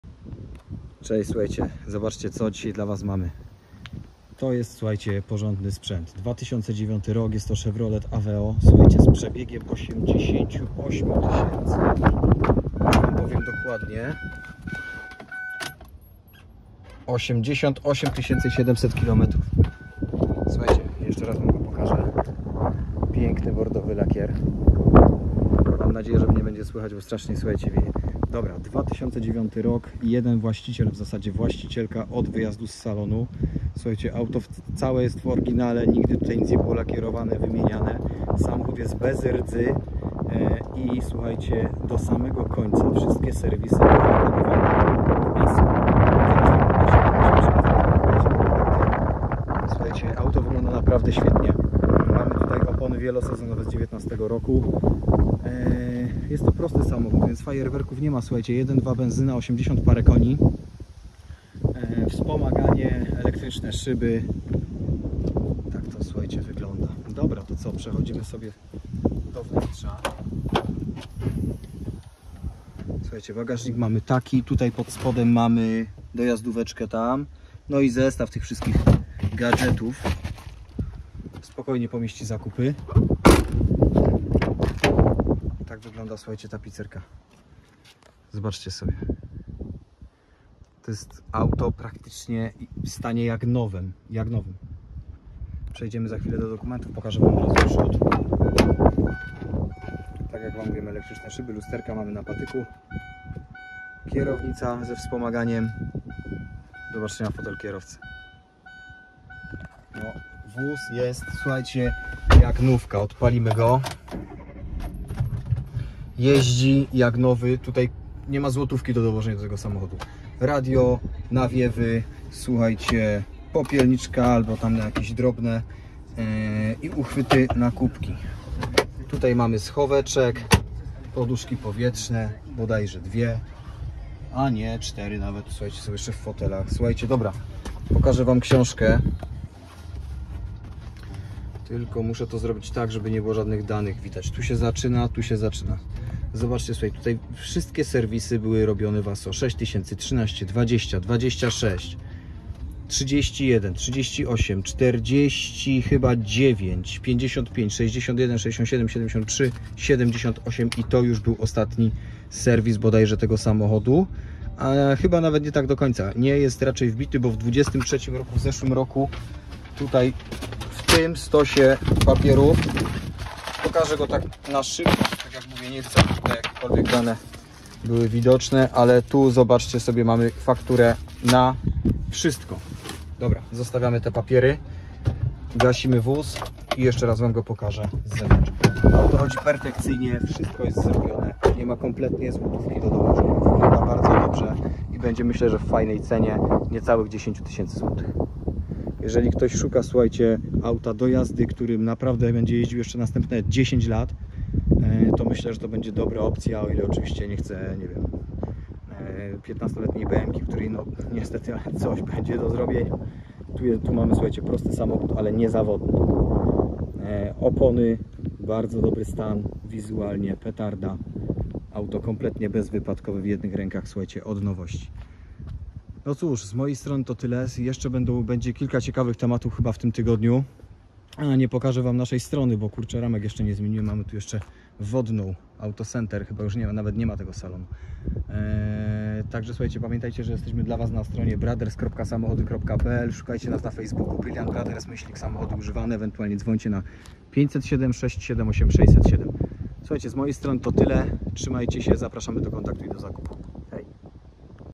CHEVROLET AVEO 1.2 BENZYNA 84KM 2009
Przedstawiamy Wam jeden z naszych samochodów na sprzedaż.

CHEVROLET AVEO
1.2 84KM, skrzynia manualna
rocznik 2009
przebieg 88 tyś km
1 właściciel od nowości
Serwis w ASO

oraz na fb :
Brilliant Brothers - samochody używane
telefon do nas:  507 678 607